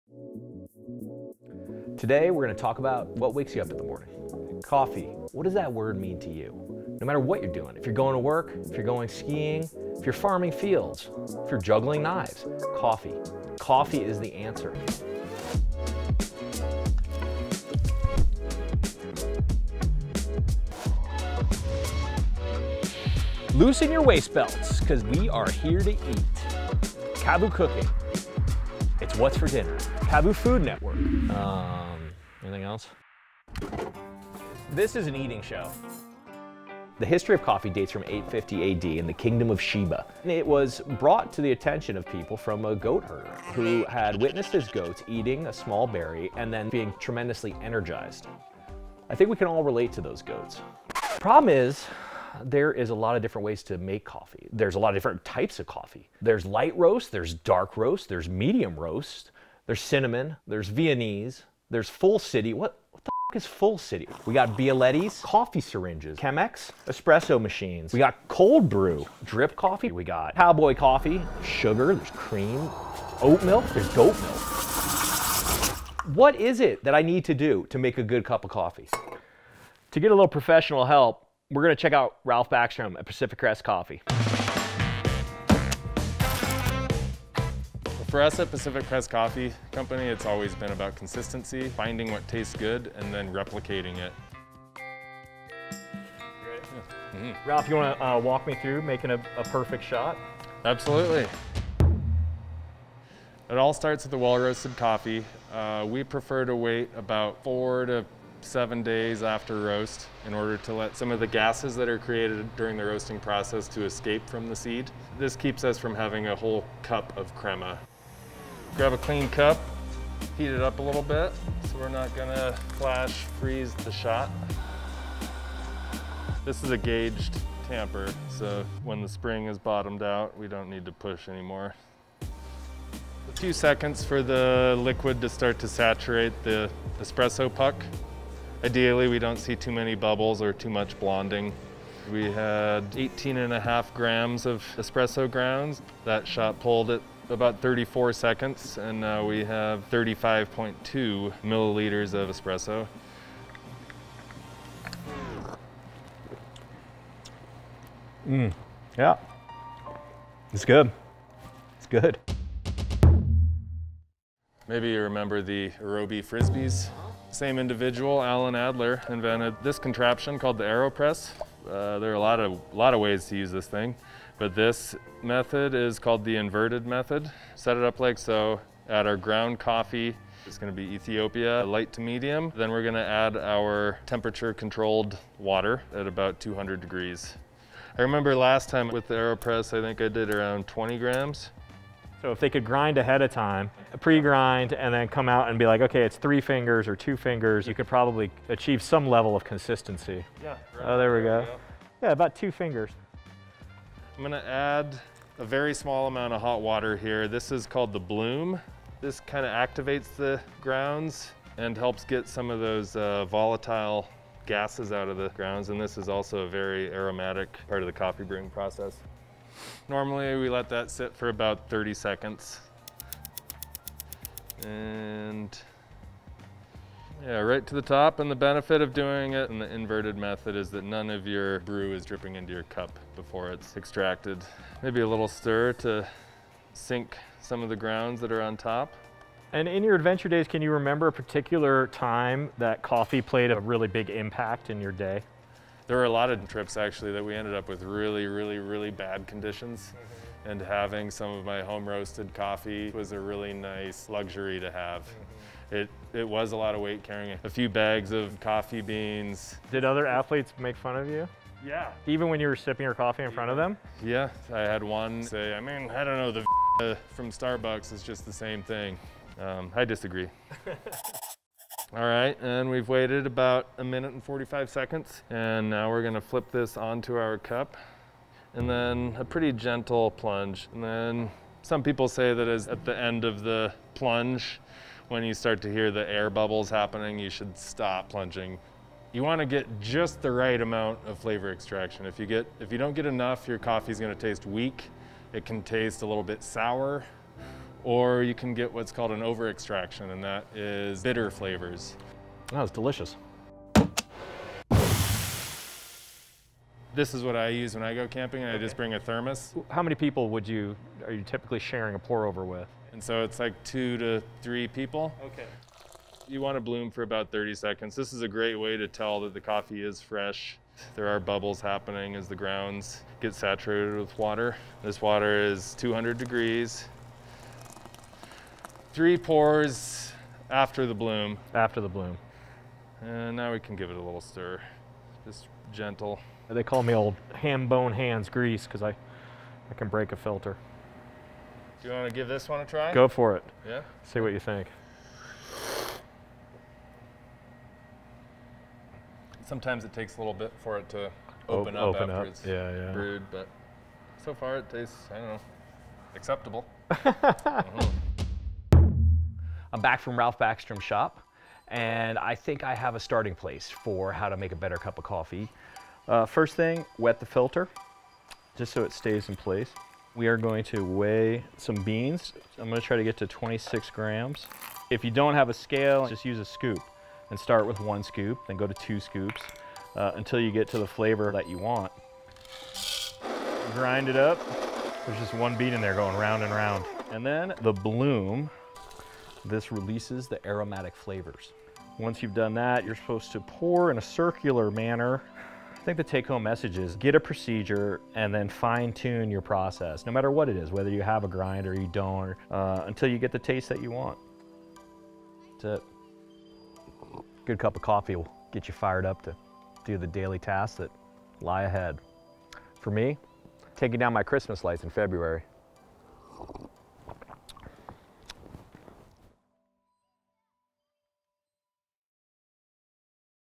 The Perfect Cup of Coffee
Cooking with KAVU | Episode 2
In this episode of Cooking with KAVU we will begin our journey into what it takes to make a great cup of coffee. Places where coffee is served often create communal meeting areas from farm towns in central Illinois to my local favorite, Pacific Crest Coffee in Truckee, California.

If you are at all like me, you thoroughly enjoy the morning ritual surrounding one of your most important first acts upon waking… making coffee. Getting correctly caffeinated is one of the most important parts of every KAVU day...Summer, Winter, Spring, or Fall...coffee always calls.